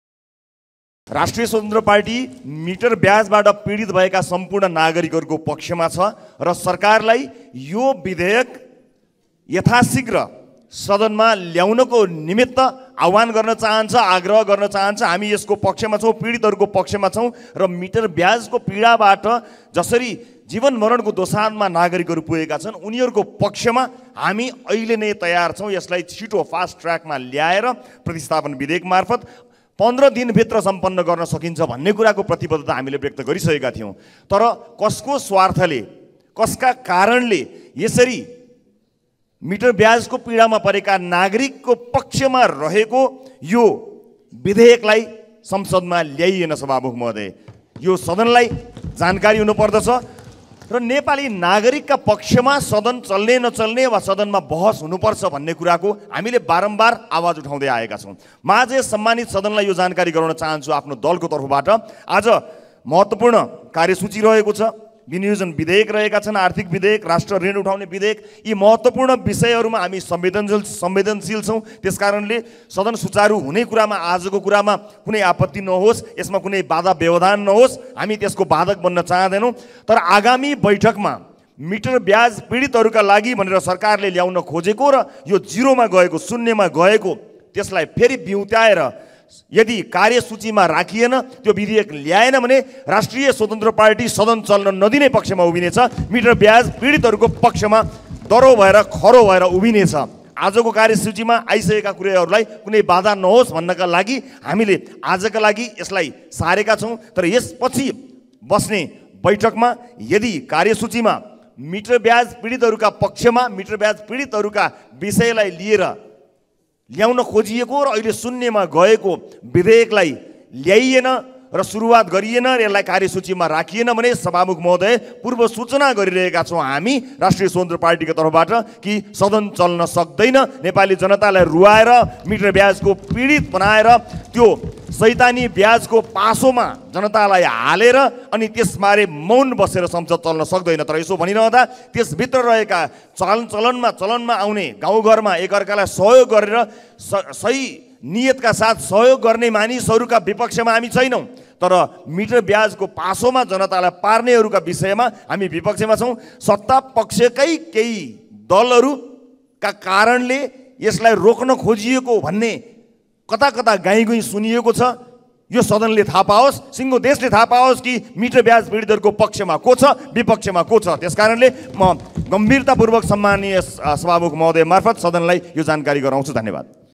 राष्ट्रिय स्वतन्त्र पार्टीले मिटरव्याजसम्बन्धी विधेयक तत्काल सदनमा नल्याए संसद अवरुद्ध गर्ने चेतावनी
राष्ट्रिय स्वतन्त्र पार्टीले मिटरव्याजसम्बन्धी विधेयक तत्काल सदनमा नल्याए संसद अवरुद्ध गर्ने चेतावनी दिएको छ ।

शुक्रबार बसेको प्रतिनिधिसभा बैठकमा रास्वपा सभापति लामिछानेले मिटरव्याजबाट पीडित नागरिकको पक्षमा आफुरहेको भन्दै मिटरव्याजसम्बन्धी विधेयक तत्काल सदनमा नल्याए संसद अवरुद्ध गर्ने चेतावनी दिनुभयो । शुन्यमा गएको उक्त बिधेयकलाई व्युताएर आगामी बैठकमा कार्यसूचीमा नराखे रास्वपा सदन चल्न नदिने पक्षमा उभिने उहाले बताउनुभयो । उहाले मिटरव्याजसम्बन्धी प्रतिस्थापन बिधेयकलाई फास्ट ट्रयाकमा पारित गर्नका लागि रास्वपा तयार रहेको पनि बताउनुभयो । लामिछानेले सत्तापक्षका केहि दलहरुका कारण मिटरव्याजसम्बन्धी कानूनलाइ रोक्न खोजिएको चर्चा चलिएको भन्दै यसको पक्षमा र बिपक्षमा को छन् भन्ने बुझ्नुपर्ने जिकिर गर्नुभयो ।

सरकारले ल्याएको मिटरब्याज सम्बन्धी अध्यादेश अन्तिम समयमा सदनमा पेश गरिएको थियो । तर सदन अवरुद्ध हुँदा उक्त अध्यादेश निष्कृय भएको थियो ।